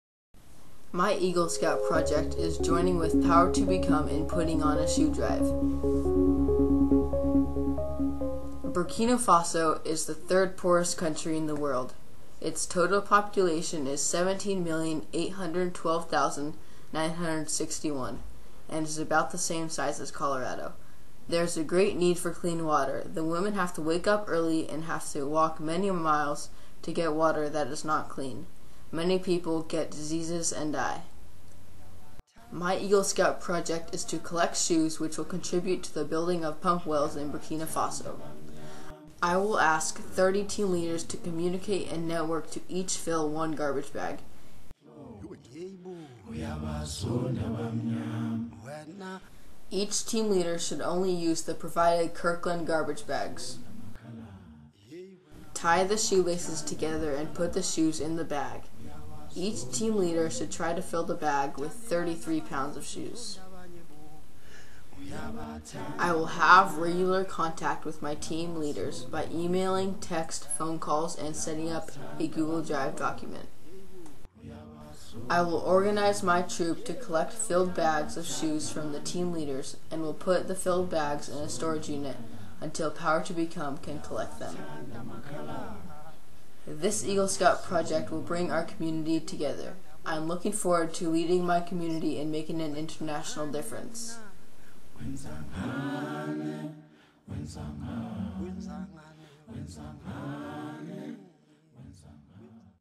Tyler Ditto - Eagle Scout Project (Power2Become Shoe Drive)
Tyler Ditto is collecting used shoes of any size and style which will then be sold to a trusted third party to be re-sold for an inexpensive amount in Africa.  Monies earned through bought shoes will be used to build Pump Wells in Burkina Faso, Africa.  Please join Tyler in his efforts to make an international difference through bringing our community together in putting on this shoe drive!!!